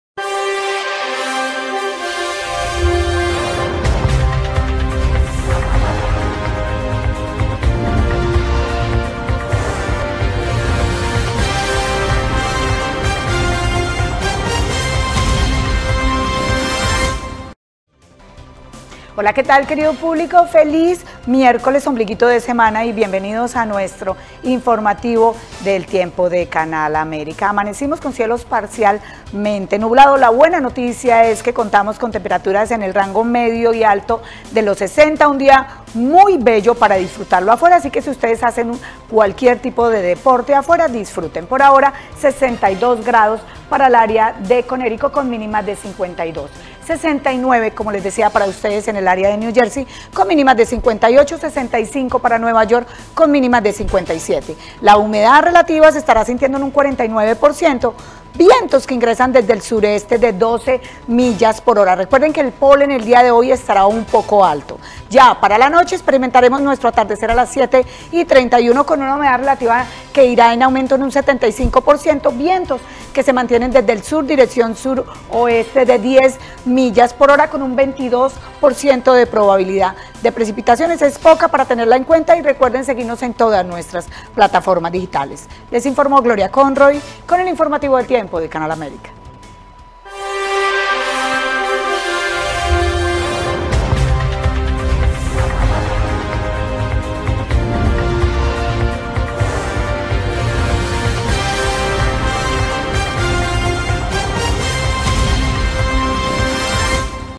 Reporte Del Tiempo 04 13 22 Con Gloria Conrroy
Enterate como estara el tiempo en el area triestatal con la bella Gloria Conrroy